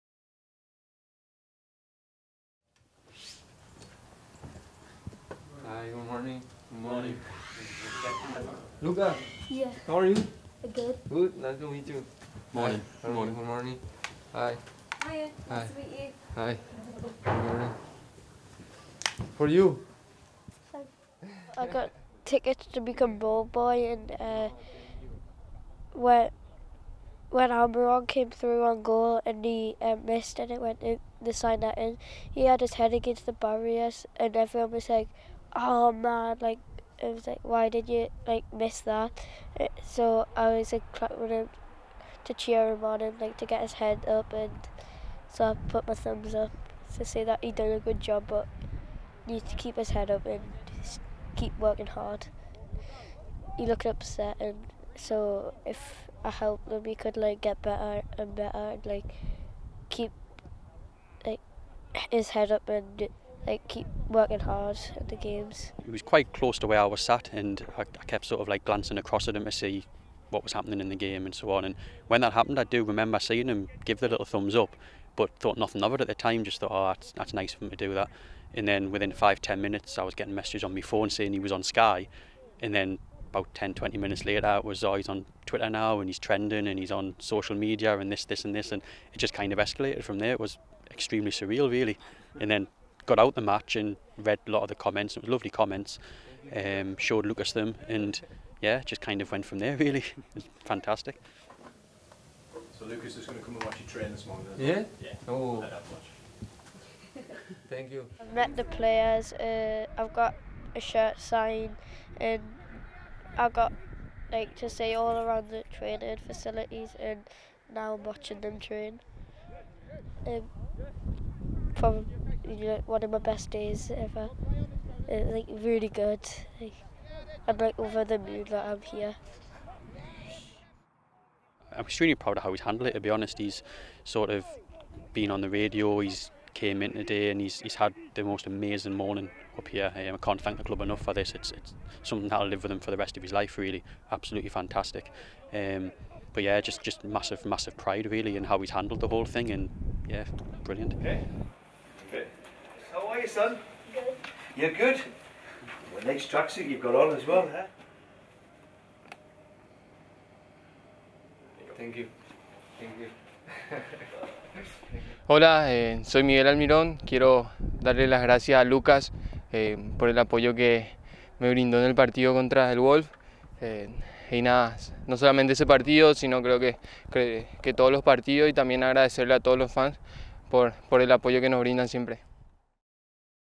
Miguel Almiron meets ballboy Lucas
Ballboy Lucas Rochford warmed the hearts of thousands after giving Miguel Almirón a 👏🏻 and 👍🏻 after a missed chance in Sunday's Premier League draw with Wolves.

The ten-year-old from South Shields was invited to the training ground today so that Almirón could thank him.